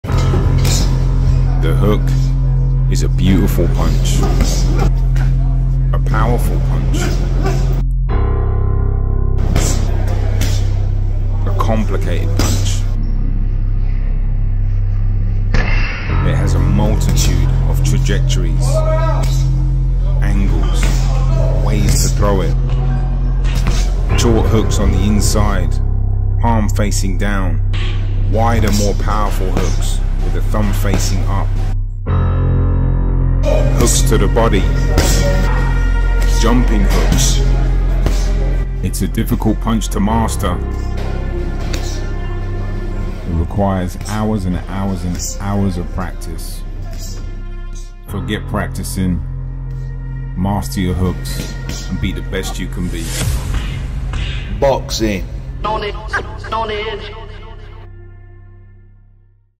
'The Hook Is A Beautiful Punch' - An Artistic Look At The Hook In BOXING
The hook is a difficult punch to master. There are may ways to throw it. Here are some examples.

Henry